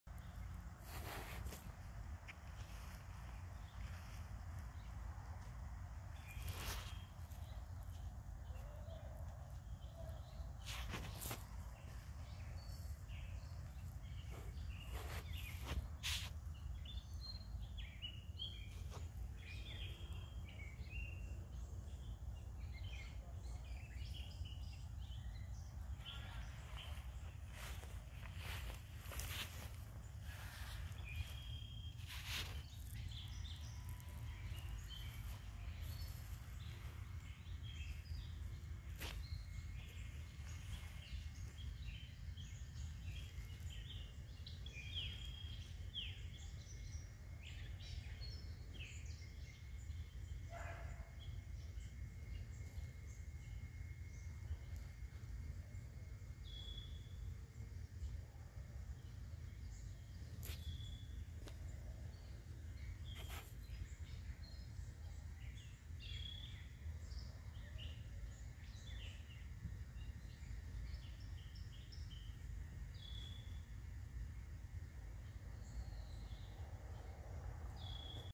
Go to bed by brushpile 7-3-19
Albino doe fawn goes to bed by the brush pile 7-3-19.